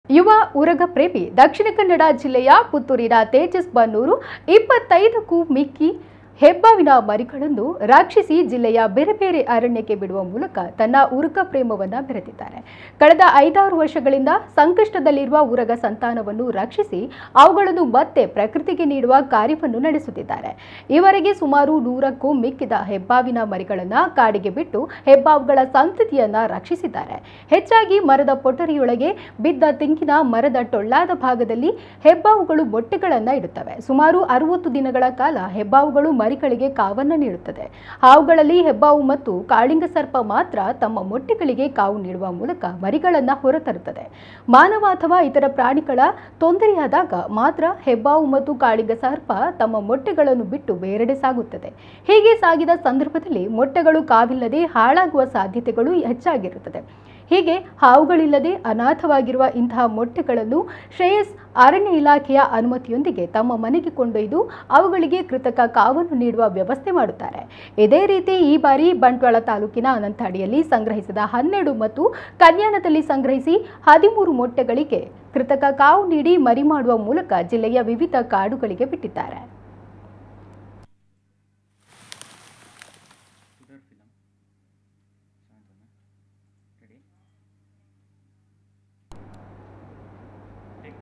ಹೆಬ್ಬಾವಿನ ಮೊಟ್ಟೆಗಳಿಗೆ ಕೃತಕ ಕಾವು ನೀಡಿ 25ಕ್ಕೂ ಮಿಕ್ಕಿದ ಮರಿಗಳ ರಕ್ಷಿಸಿದ ಪುತ್ತೂರಿನ ಸ್ನೇಕ್ ತೇಜಸ್..!
NAMMA KUDLA news 24x7
ತಾಜಾ ಸುದ್ದಿಗಳಿಗೆ  ನಮ್ಮ ಕುಡ್ಲ whatapp ಗ್ರೂಪ್‌ಗೆ  ಸೇರಿ..